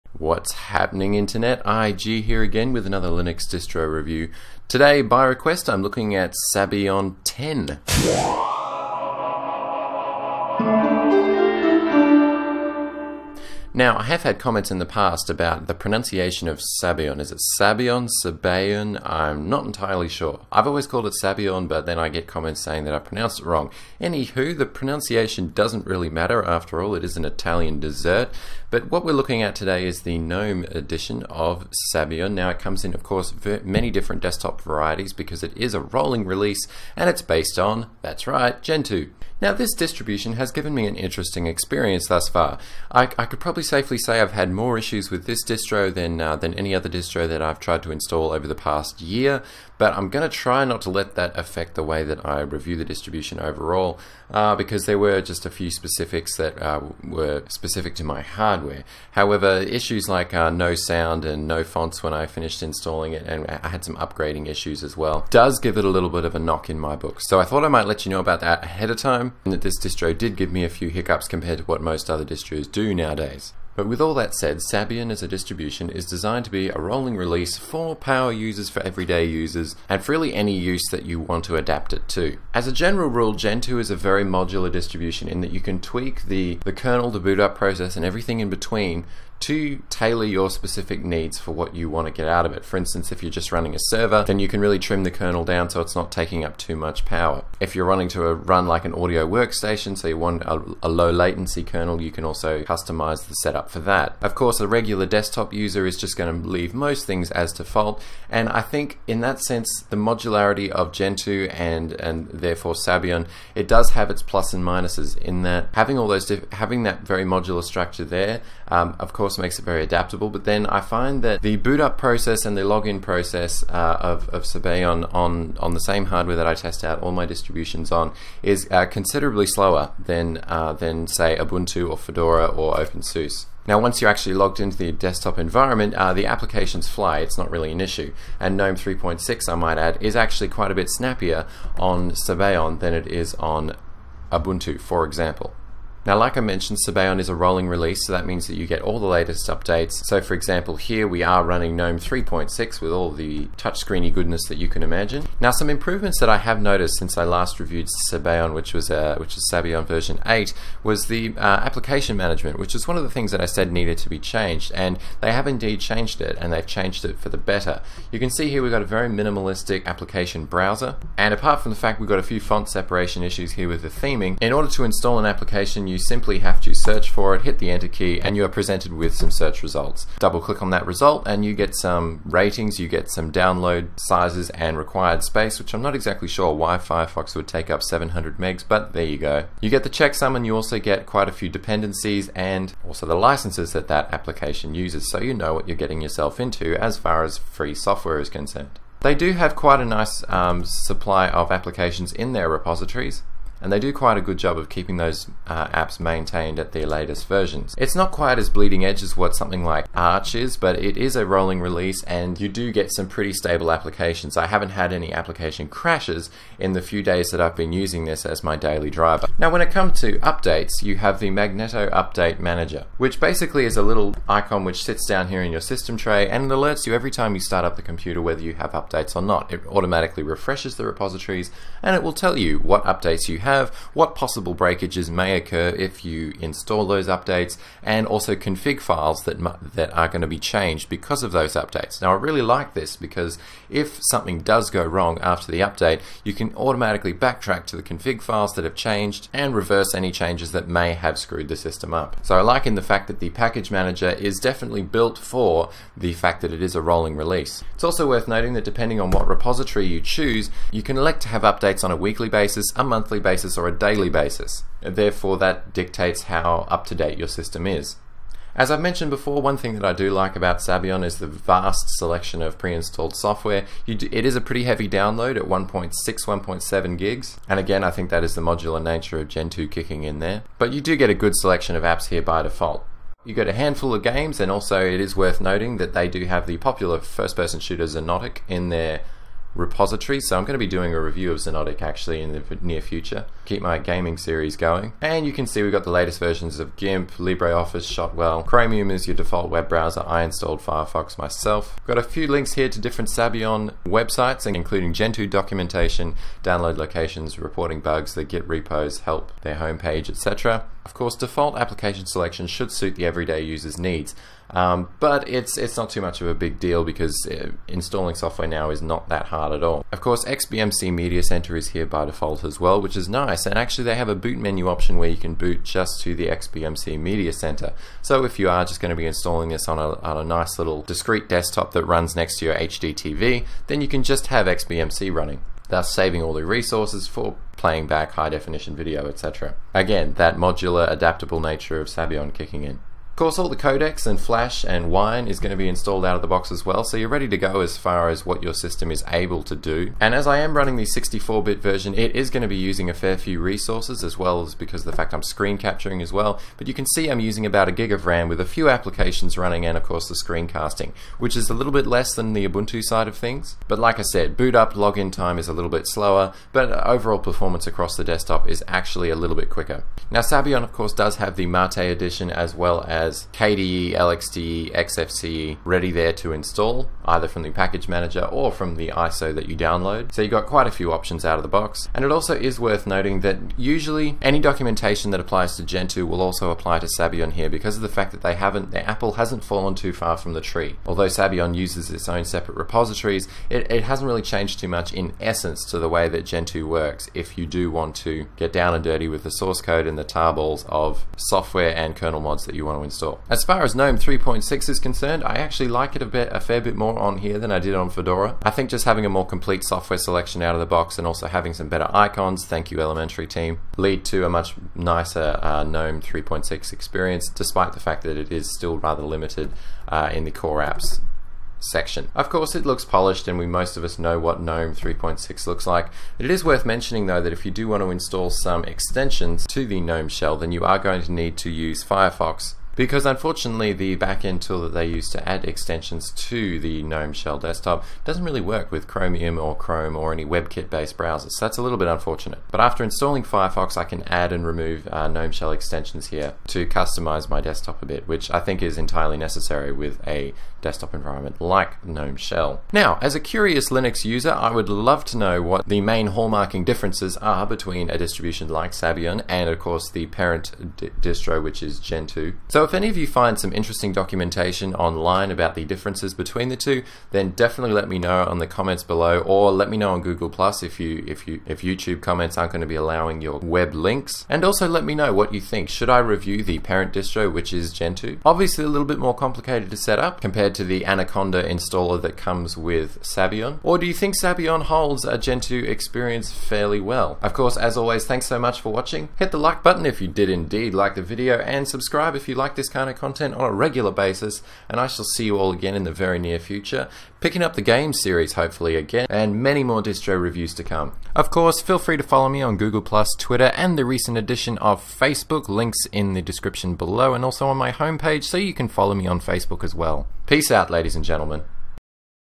Sabayon 10 Review - Linux Distro Reviews
==Should I review the mothership of Sabayon (Gentoo)? If so, why?==
A review of the modular rolling release distribution Sabayon. Based on Gentoo, this distro has a very adaptable nature with lots of preinstalled apps, good documentation, and a wide variety of scenarios and software supported in the repositories. Whether you're looking for a server, a gaming OS, a media station, or a regular desktop Sabayon will be what you need...nothing more, nothing less.